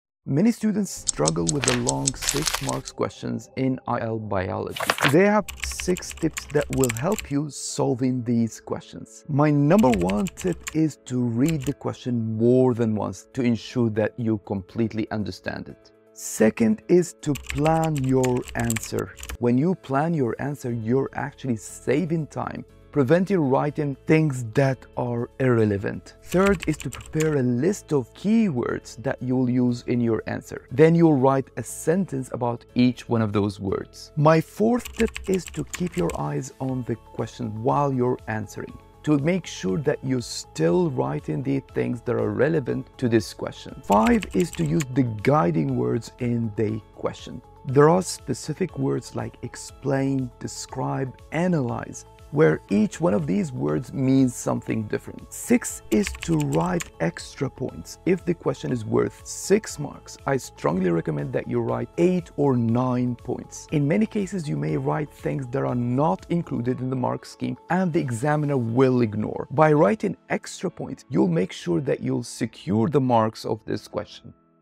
How to solve the long 6 mark questions for IAL Biology Exams?
Try a free class for IAL biology 👇

chem-bio.info/free-ial-biology-classes

You can also join us live every week on Zoom 📺

You can also get the physical book from Amazon 📕

🎓📚 **Ace Your IAL Biology Exams with These 6 Tips!** 📚🎓

Struggling with those daunting six-mark questions in your IAL Biology exams? You're not alone! But don't worry, because today, I've got six powerful tips to help you tackle these questions with confidence! 💪

1️⃣ **Read Twice, Answer Once**: Make sure you read the question more than once to fully grasp what it's asking. Understanding is key! 🔍

2️⃣ **Plan Before You Write**: Planning your answer can actually save you time and prevent you from writing irrelevant information. ⏳📝

3️⃣ **Prepare Your Keywords**: Have a list of key terms ready. Write a sentence about each one to ensure you include them in your answer. 🗝️✍️

4️⃣ **Stay Focused**: Keep your eyes on the question as you write to ensure everything you include is relevant. 👀✔️

5️⃣ **Use Guiding Words**: Pay attention to directive words like "explain," "describe," and "analyze." Each one requires a different approach. 🧠🔍

6️⃣ **Add Extra Points**: For a question worth six marks, aim to write eight or nine points. This way, you maximize your chances of scoring full marks! 🏆✏️

Follow these tips, and you'll be well on your way to mastering those six-mark questions in your IAL Biology exams! 🌟📖